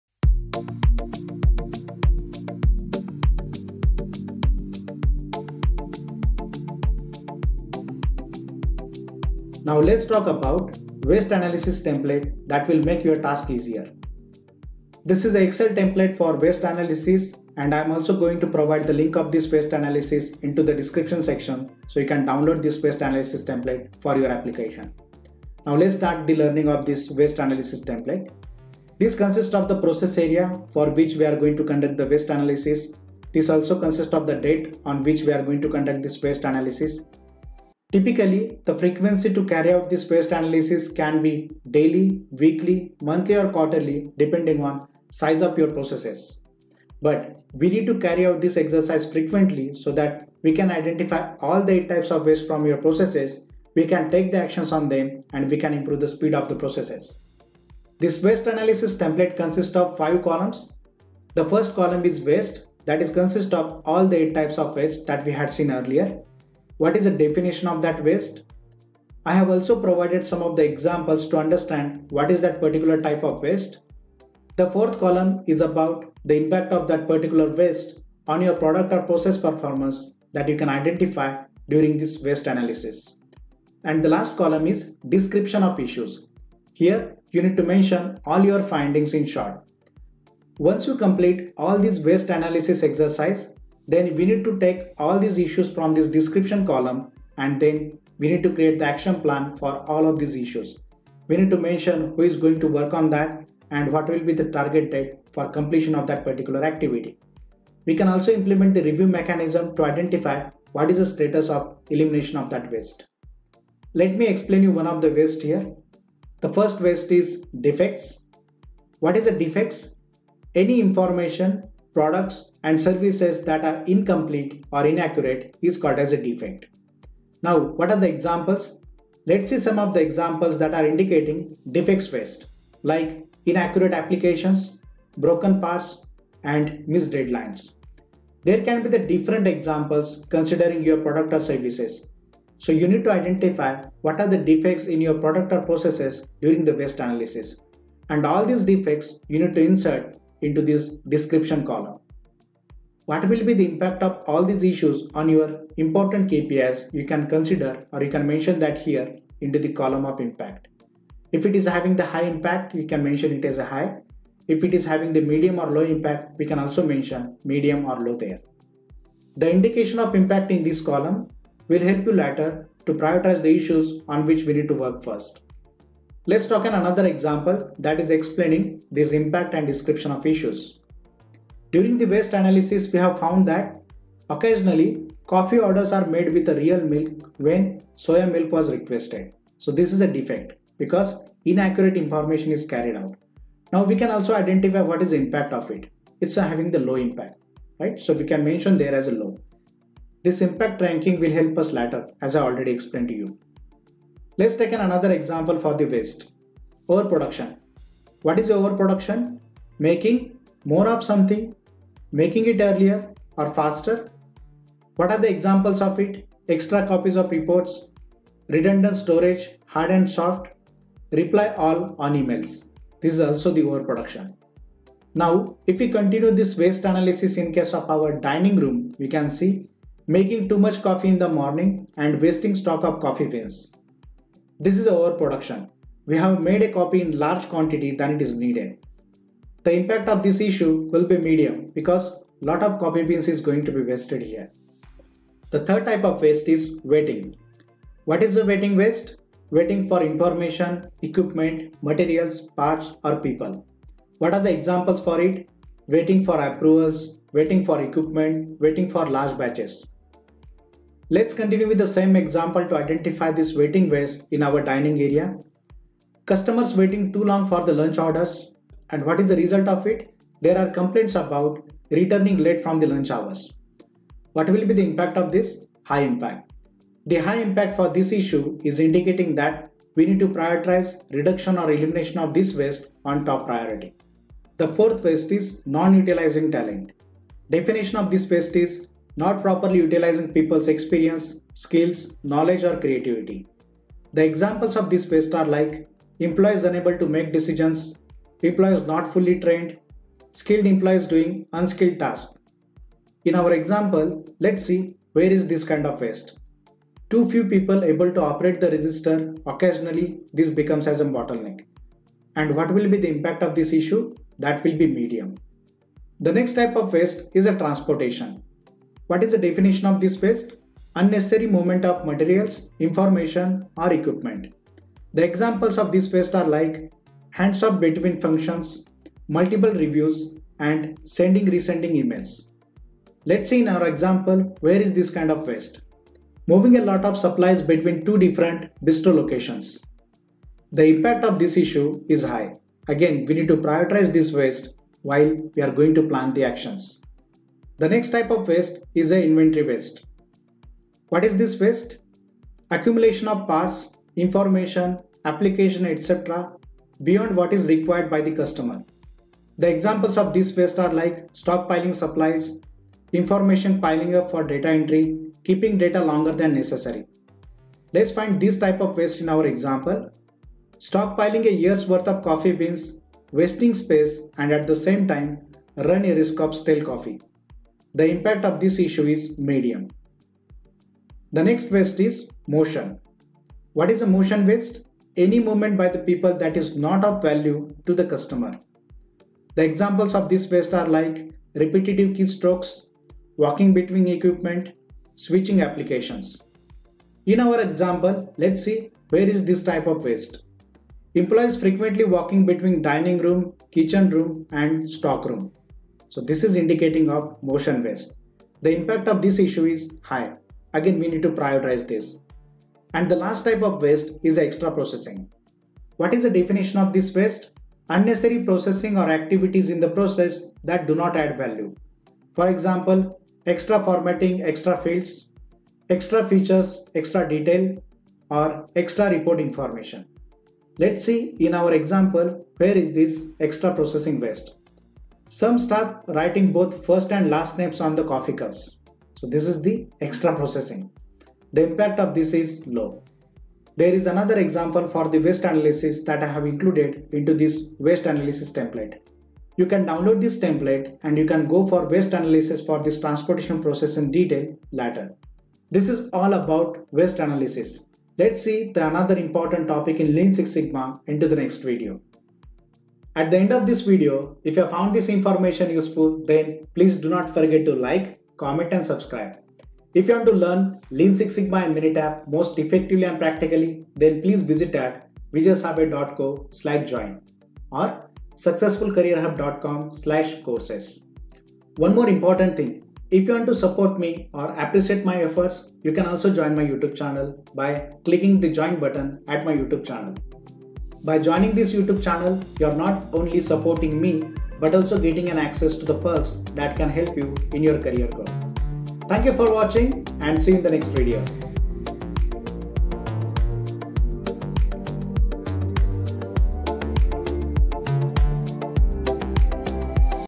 How To Find 8 Types Of Waste With Example: Waste Analysis PART 2
To learn Lean Six Sigma and Minitab most effectively and practically,
Hello Friends,
           In this video, you will learn How To Find 8 Types Of Waste With examples. This video provides you the guidelines to find all 8 types of waste in your processes.

It consists of the following topics:
✅ Introduction of Waste Analysis Template
✅ Frequency to conduct Waste Analysis
✅ Waste Analysis Excel Template to find all 8 Types of Waste in LEAN
✅ Identify DEFECT Waste in Process with Example
✅ Identify OVERPRODUCTION Waste in Process with Example
✅ Identify WAITING Waste in Process with Example
✅ Identify NON-UTILIZED TALENT Waste in Process with Example
✅ Identify TRANSPORTATION Waste in Process with Example
✅ Identify INVENTORY Waste in Process with Example
✅ Identify MOTION Waste in Process with Example
✅ Identify EXTRA PROCESSING Waste in Process with Example
✅ One More Example For Waste Analysis Exercise (Waste Analysis Template)
✅ Learn Lean Six Sigma and Minitab Most Effectively and Practically
           I am sure you will like it.

0:00 Introduction of Waste Analysis Template
0:37 Frequency to conduct Waste Analysis
0:58 Explanation of Waste Analysis Excel Template
1:54 Identify DEFECT Waste in Process with Example
3:27 Identify OVERPRODUCTION Waste in Process with Example
4:05 Identify WAITING Waste in Process with Example
4:44 Identify NON-UTILIZED TALENT Waste in Process with Example
5:15 Identify TRANSPORTATION Waste in Process with Example
5:48 Identify INVENTORY Waste in Process with Example
6:21 Identify MOTION Waste in Process with Example
6:53 Identify EXTRA PROCESSING Waste in Process with Example
7:29 One More Example For Waste Analysis Exercise (Waste Analysis Template)
7:50 Learn Lean Six Sigma and Minitab Most Effectively and Practically